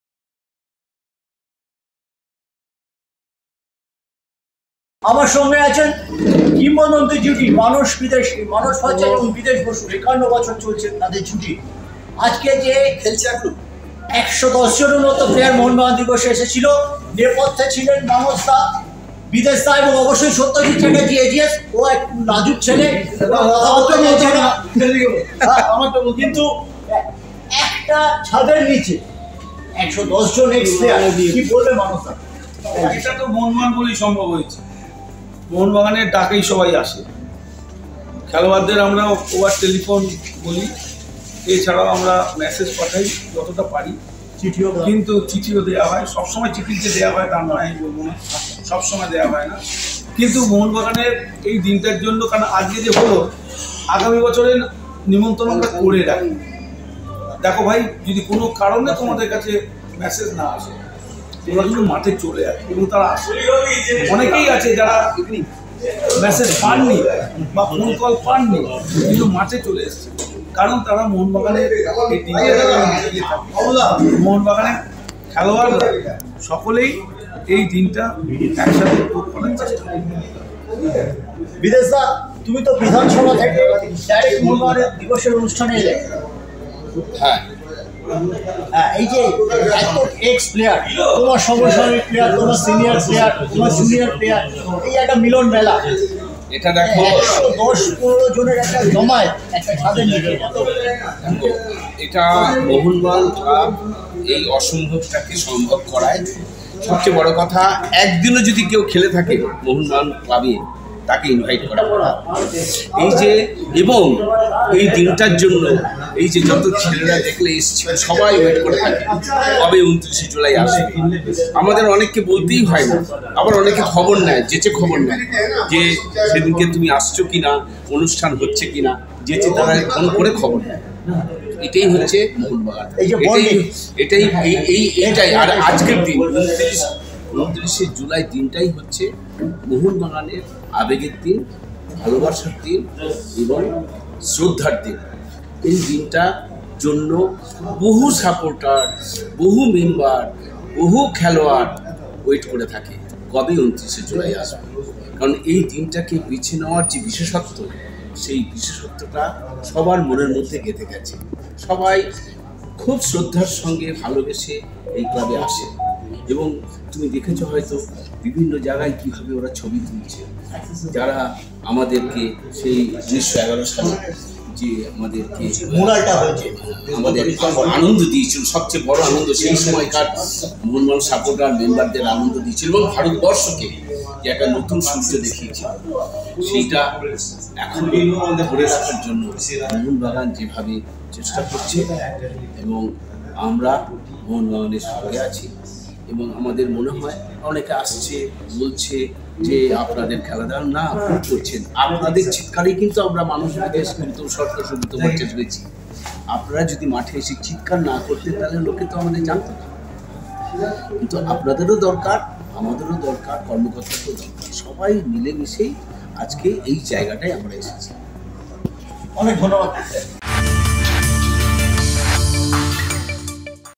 মোহনবাগান দিবসে ড্রেসিংরুমে বসে মানস-বিদেশের সঙ্গে সত্যজিতের খুনসুটি | Spot On News
মোহনবাগান দিবসে ড্রেসিংরুমে বসে মানস-বিদেশের সঙ্গে সত্যজিতের খুনসুটি